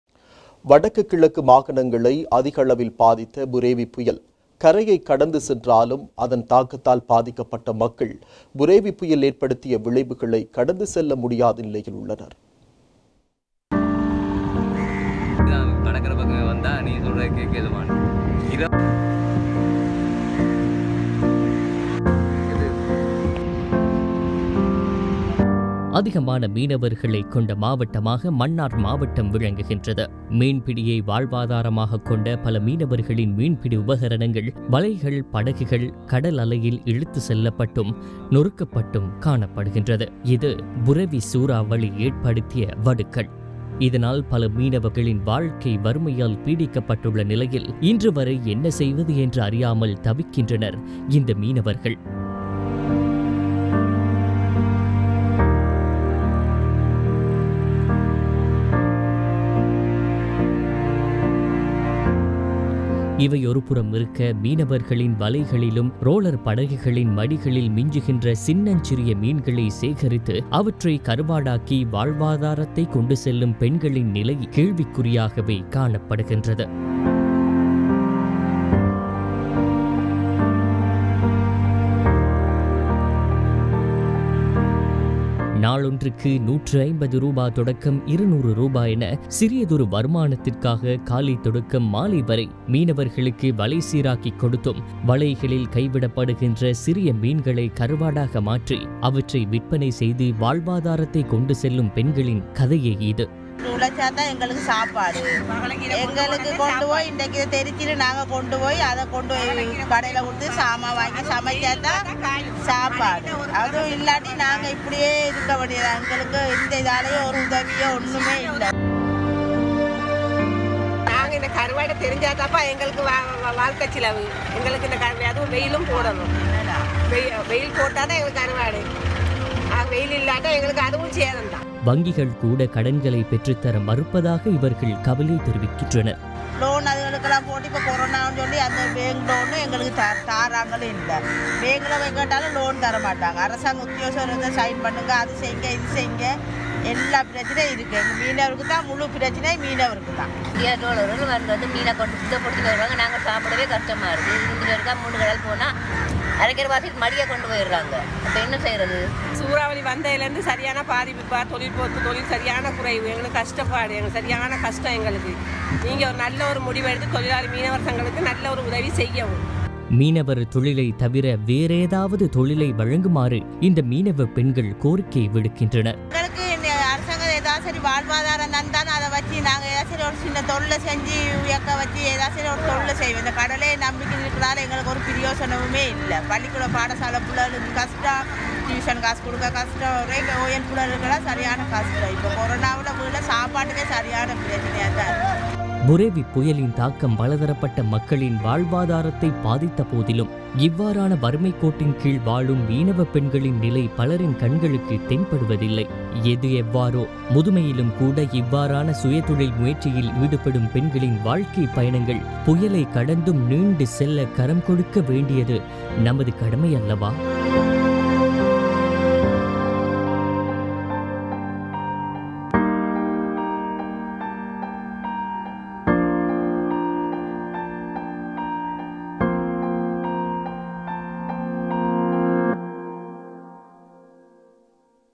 புரேவி புயல் கரையை கடந்து சொன்றாலும் மக்கள் அதன் விளைவுகளை கடந்து செல்ல முடியாத நிலையில் உள்ளனர்.
புரேவி புயல் கரையை கடந்து சொன்றாலும் மக்கள் அதன் விளைவுகளை கடந்து செல்ல முடியாத நிலையில் உள்ளனர்.  | UTV Tamil HD